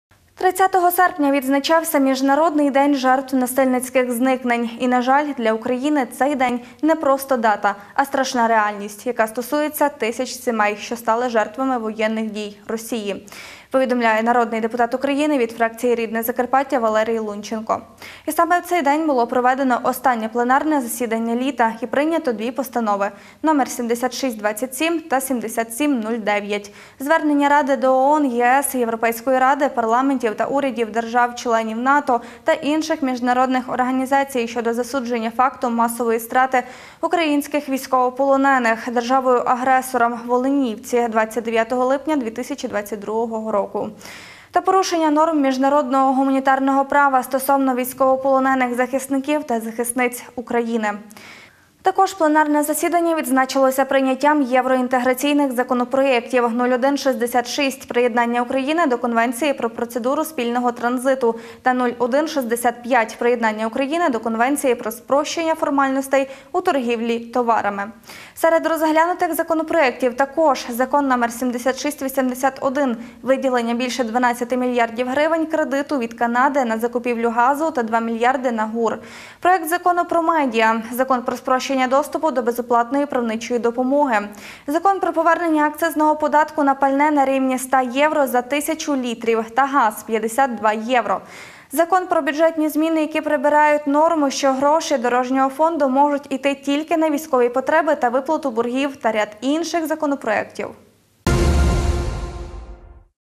Останнє літнє пленарне засідання ВРУ
30 серпня відзначався Міжнародний день жертв насильницьких зникнень і, на жаль, для України цей день не просто дата, а страшна реальність, яка стосується тисяч сімей, що стали жертвами воєнних дій росії – повідомляє народний депутат України від фракції «Рідне Закарпаття» Валерій Лунченко